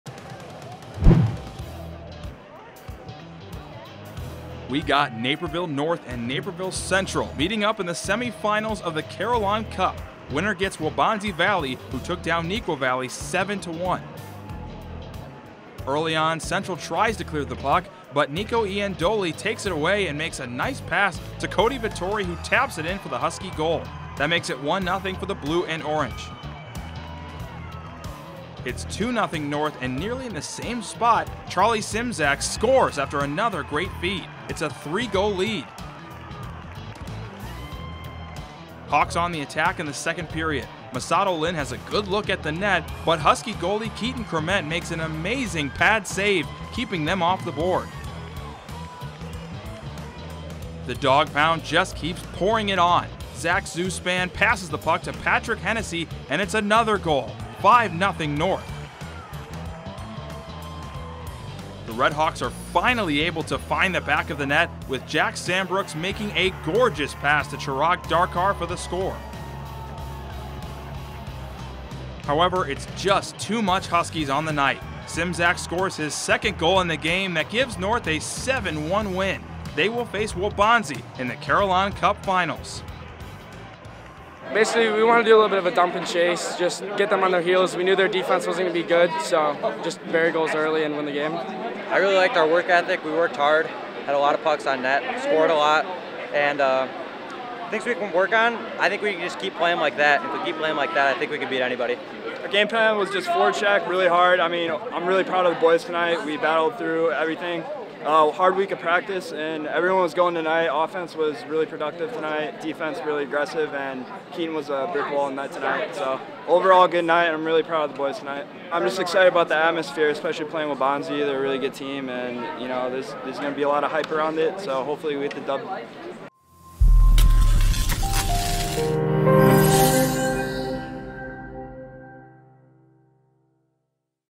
Hockey Carillon Cup Semifinals Naperville North vs. Naperville Central 11.18.21
The Carillon Cup semifinals take place at All Seasons where Naperville North faces Naperville Central.
We got Naperville North and Naperville Central meeting up in the semifinals of the Carillon Cup. Winner gets Waubonsie Valley who took down Neuqua Valley 7-1.

First Period

Early on, Central tries to clear the puck, but Nico Iandoli takes it away and makes a nice pass to Cody Vittori who taps it in for the Huskie goal. That makes it 1-0 for the blue and orange.

It’s 2-0 North and nearly in the same spot, Charlie Szymczak (Sim-Zack) scores after another great feed. It’s a three-goal lead.

Second Period

Hawks on the attack in the second period. Masato Lin has a good look at the net, but Huskie goalie Keaton Crement makes an amazing pad save keeping them off the board.

The Dawg Pound just keeps pouring it on. Zach Zuspann passes the puck to Patrick Hennessy and it’s another goal. 5-0 North.

The Redhawks are finally able to find the back of the net with Jack Sambrooks making a gorgeous pass to Chirag Dharkar for the score.

Third Period

However, it’s just too much Huskies on the night. Szymczak scores his second goal in the game that gives North a 7-1 win. They will face Waubonsie in the Carillon Cup finals.